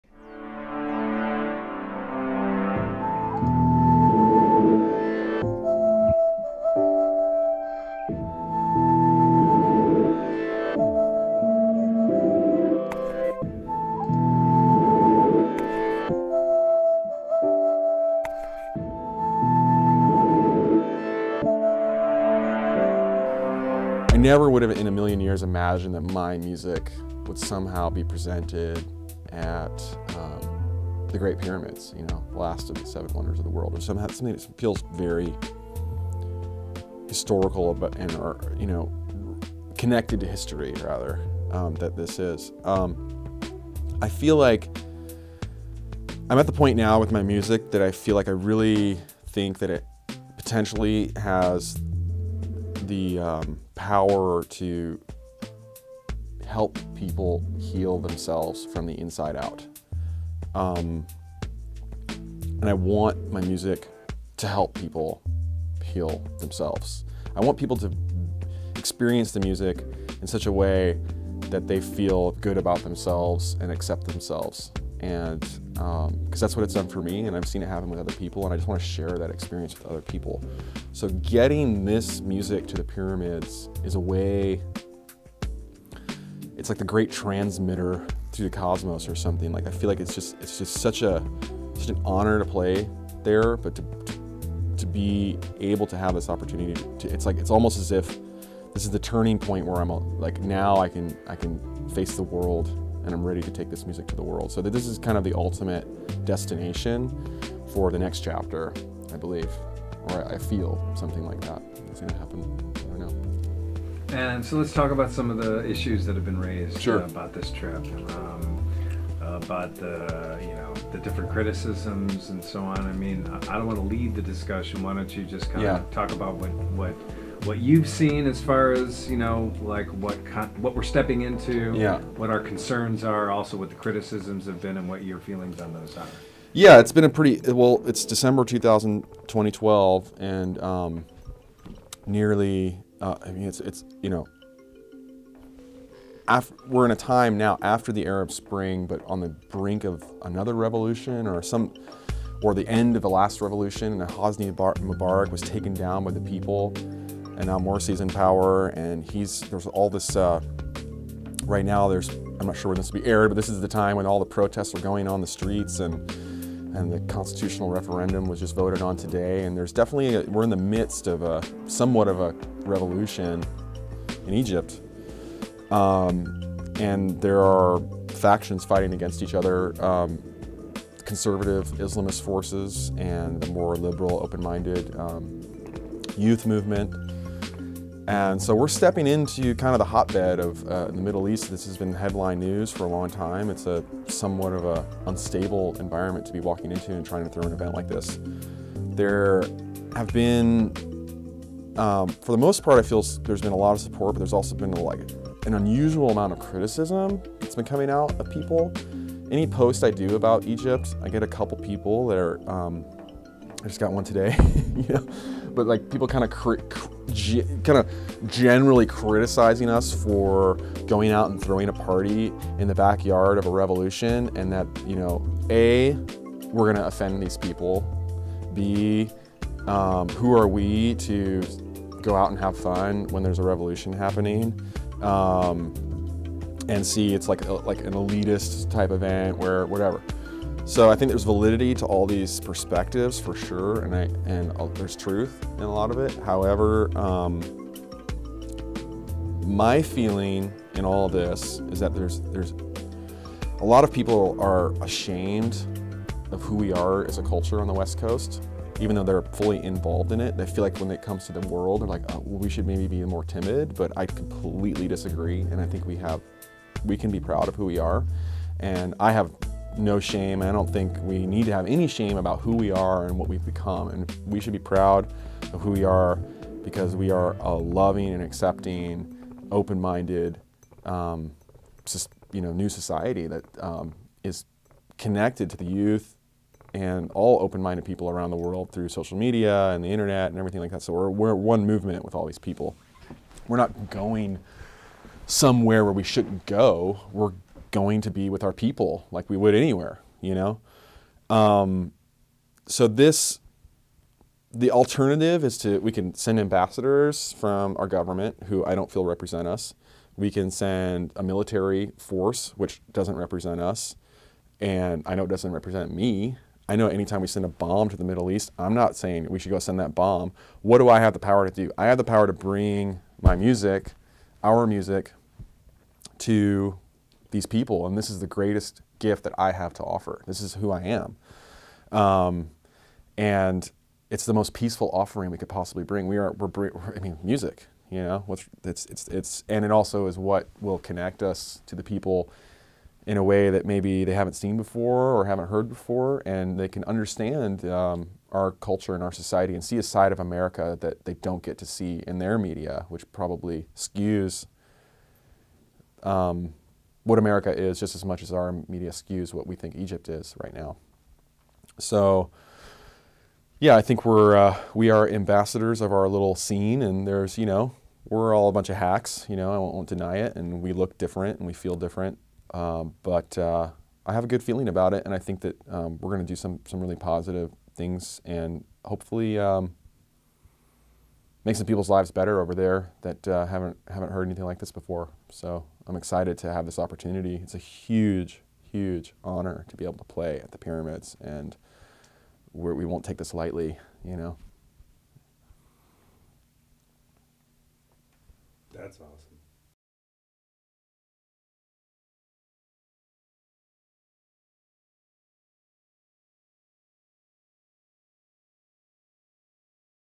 Random Rab on playing at the pyramids in Egypt
On the eve of The Great Convergence 2012 solstice celebration on the Giza plateau in Egypt, Random Rab sits down with Charles Shaw to discuss playing his music at the Great Pyramids.